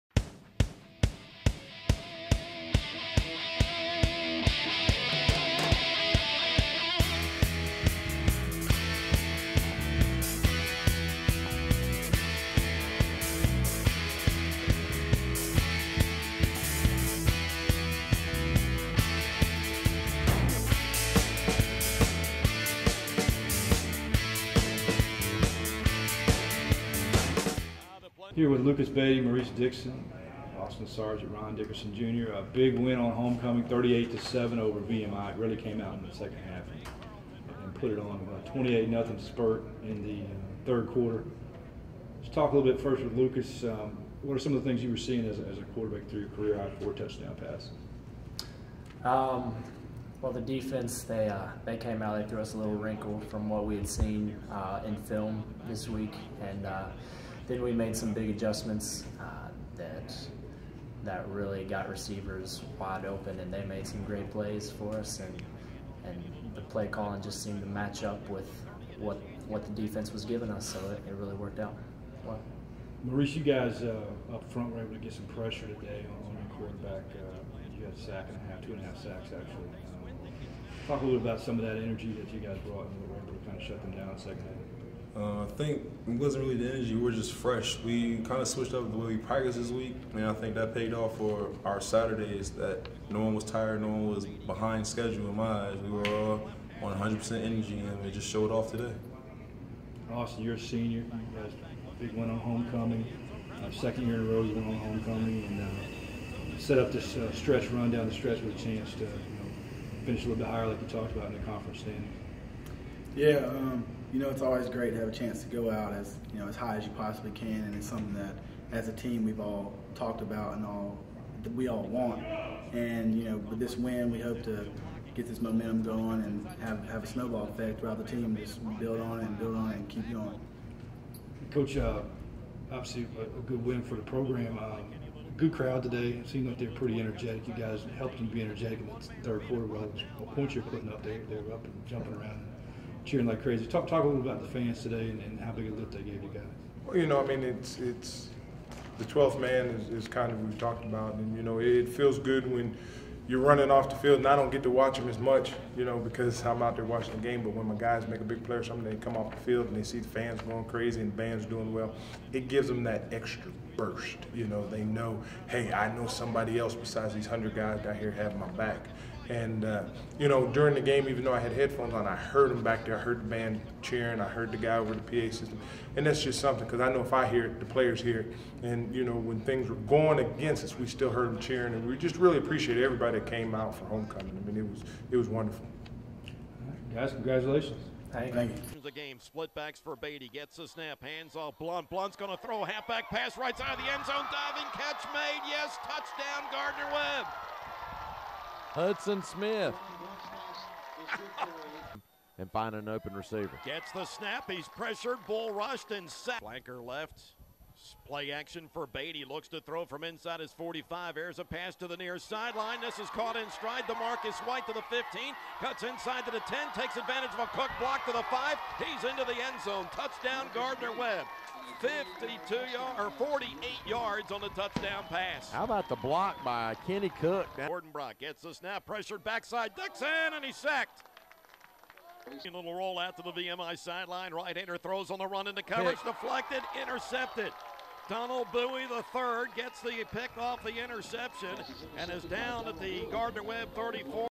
Gardner-Webb Football: VMI Highlights and Postgame Comments (10-27-12)
Earlier today, Gardner-Webb defeated VMI by a score of 38-7 on Homecoming Weekend.  Check out highlights from today's game, as well as postgame comments from Lucas Beatty, Maurice Dickson, Austin Sargent, and head coach Ron Dickerson, Jr.